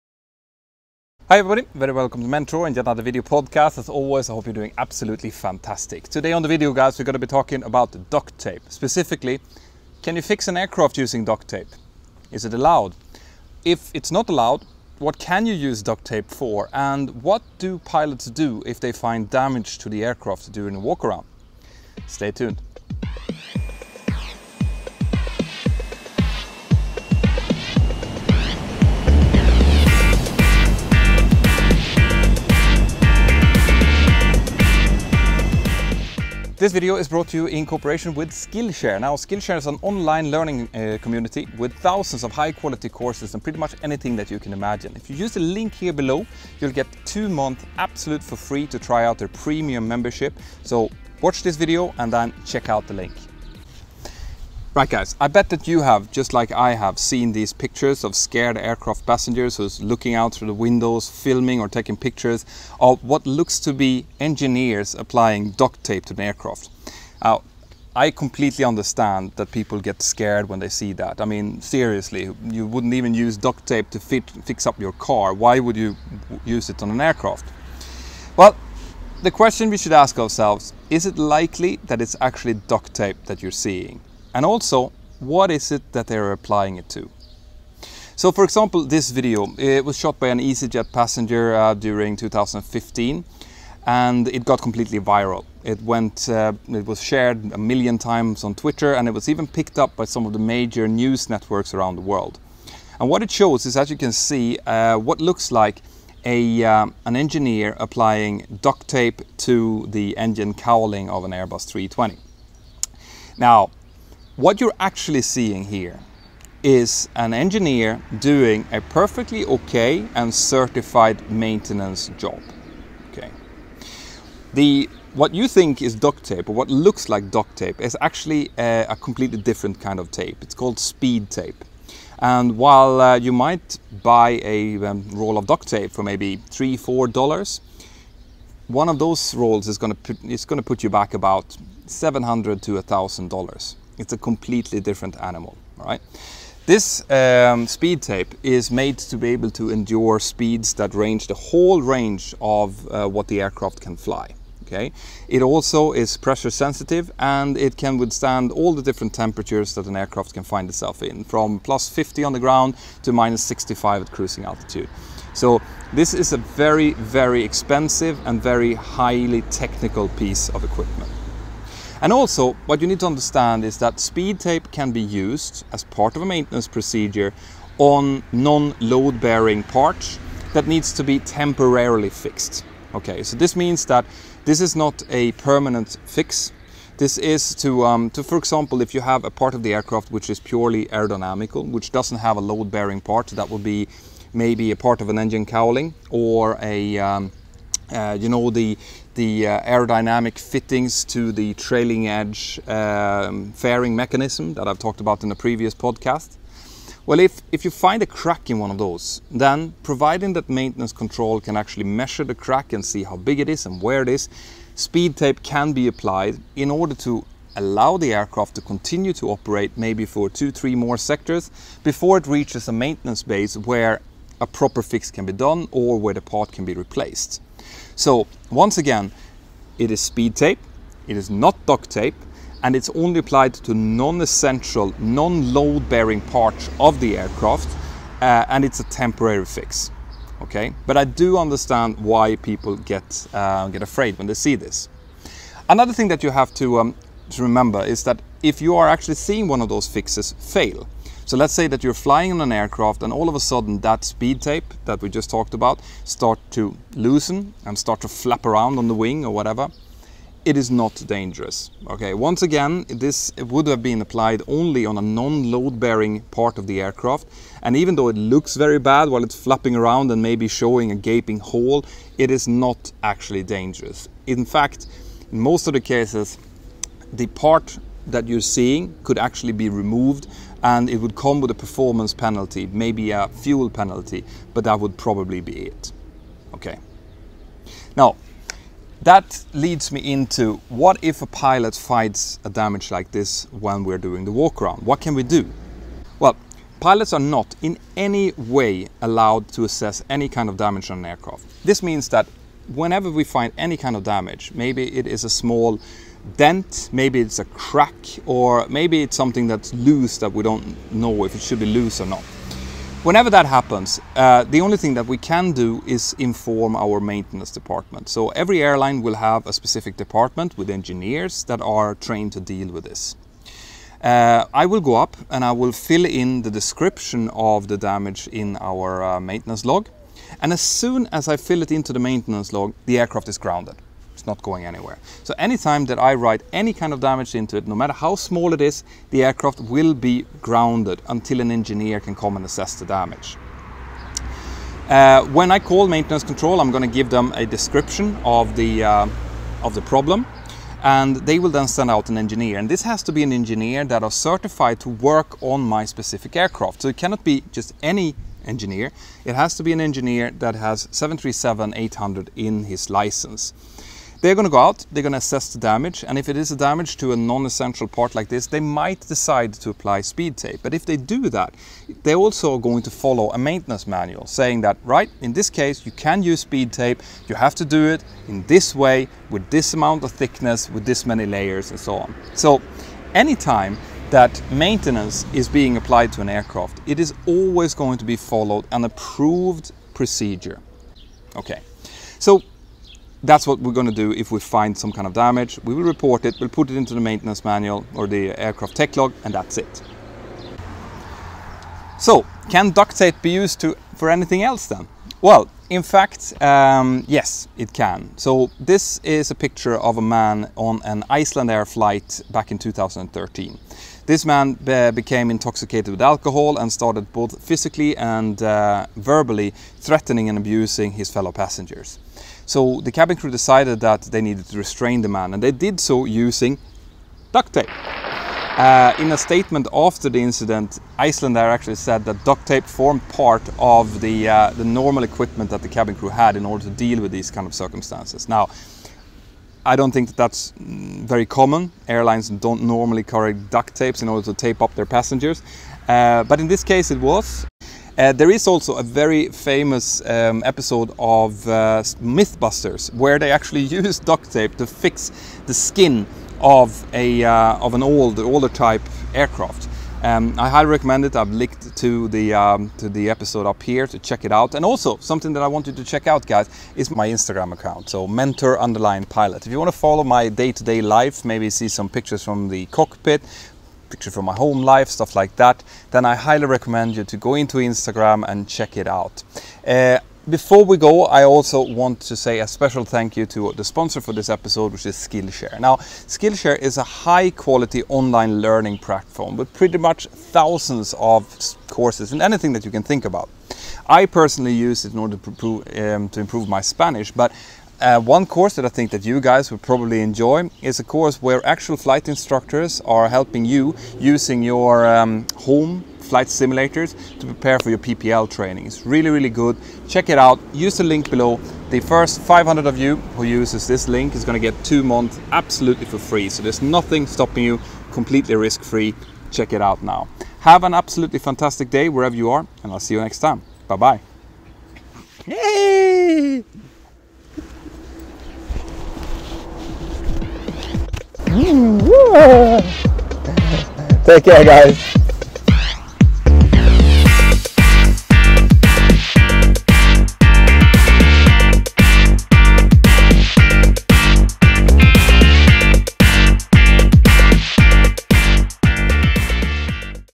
Fixing aircraft with DUCT TAPE?! Mentour Pilot explains.
Have you ever seen pictures or videos of Aircraft engineers seemingly mending an aircraft using Duct-tape? Is that possible, is it legal and is it REALL duct tape that the engineers are using?
I will also be talking about what pilots do if they encounter any kind of damage to the aircraft.
I hope this episode will explain some aircraft maintenance procedures for you and increase your confidence in those.

If you want to chat with me and get all Mentour related news, then download my FREE app. 👇🏻

A huge thank you to the makers of todays featured videos. Check out the full videos below!